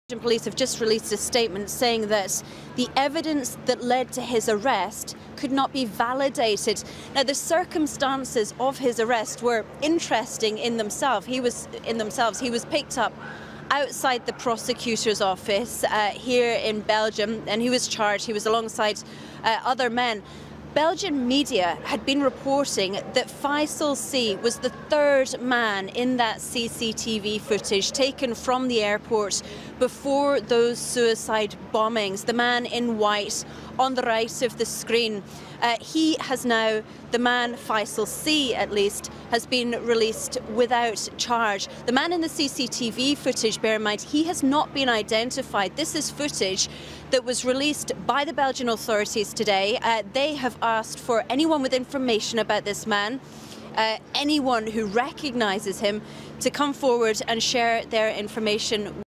Brussels attacks Terror suspect released by police
A man who was charged with "terrorist murder" over the Brussels attacks has been released by Belgian police.
Belgium's federal prosecutor's office now says it does not have enough evidence to continue holding the man, named only as Faycal C.